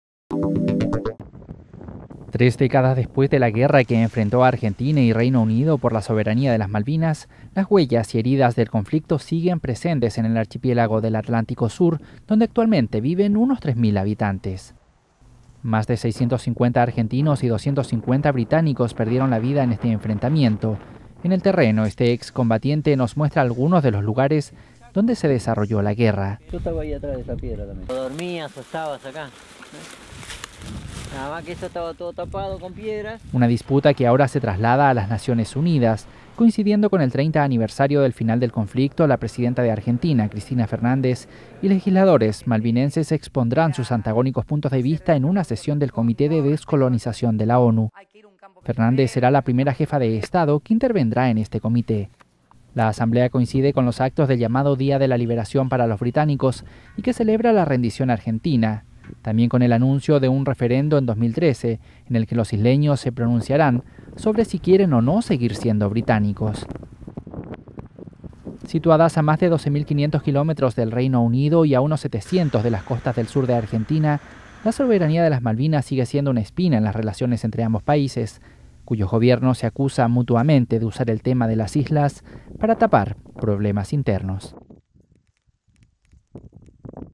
Treinta años después, la pelea por las Malvinas es de palabras y en la ONU.wmv
La presidenta de Argentina, Cristina Fernández, y legisladores malvinenses expondrán sus antagónicos puntos de vista sobre la soberanía de las Malvinas hoy en una sesión del Comité de Descolonización de la ONU cuando se cumplen 30 años del fin de la guerra argentino-británica.